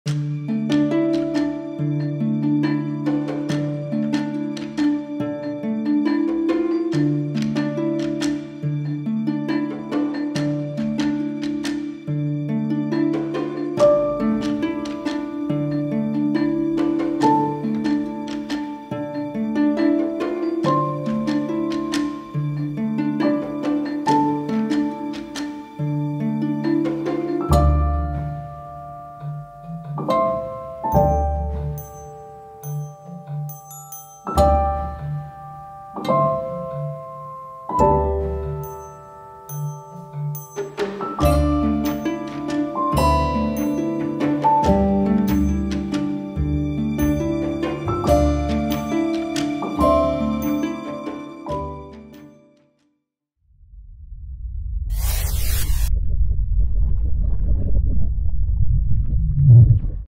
Medieval belt buckle found in Czech Republic may be from unknown pagan cult
A belt buckle featuring a snake devouring a frog was found in the Czech Republic. Full Story

"Archaeologists thought the bronze belt fitting from Lány in the Czech Republic was unique, but they've since learned that at least three others have been found, including in Hungary and in southern Germany," according to Live Science.

Credit: Live Science | imagery courtesy: MU Faculty of Arts | edited by Steve Spaleta